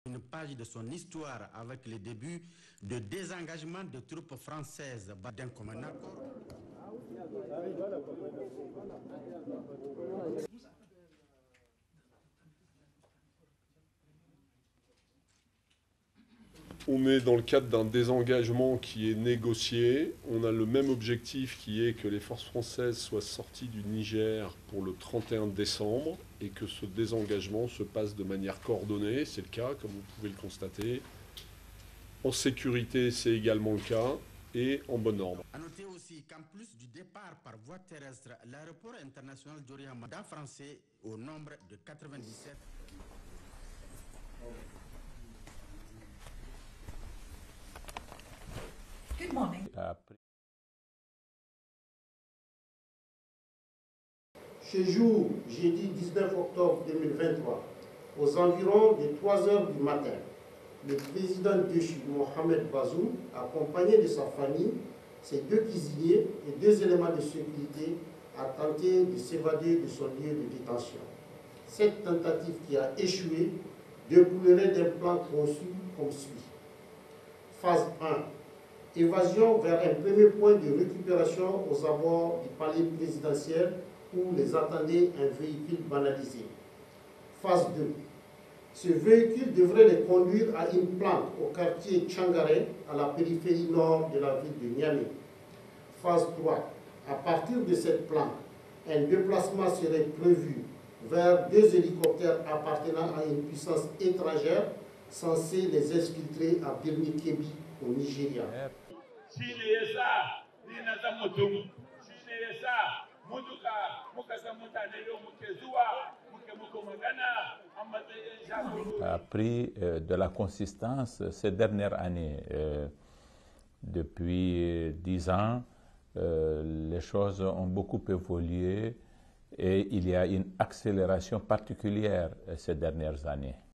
French forces disengage from Niger
(20 Oct 2023) The commander of French forces in the Sahel region of Africa said French troops were "in the context of a negotiated disengagement" from Niger. (October 20).